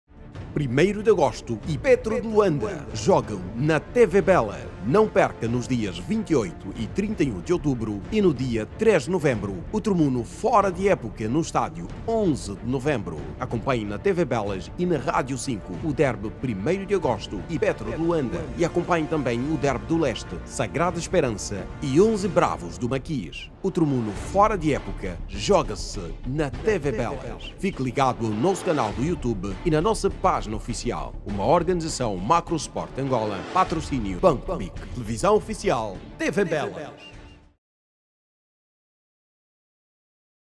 TRUMUNU FORA DE ÉPOCA
A TV Belas é o canal oficial para a transmissão dos jogos do Torneio quadrangular “TRUMUNU FORA DE ÉPOCA”, que inicia nesta quarta-feira, no estádio 11 de novembro.
A emissão das 6 partidas, que vão ser disputadas nos dias 28 e 31 de outubro e 3 de novembro, será feita através do canal oficial da TV Belas no youtube

A parceria entre a organização do torneio e a TV Belas surgi da necessidade da transmissão das partidas de futebol através de uma plataforma, que permita aos telespectadores acompanharem e viverem as emoções do jogo em directo, uma vez que ainda está proibida a presença de público nos estádios.
Recordar que o “Trumuno fora de época”, em que participam o Bravos do Maquis, Sagrada Esperança, Petro de Luanda e 1º D´Agosto, tem como objectivo preparar as equipas para as afrotaças.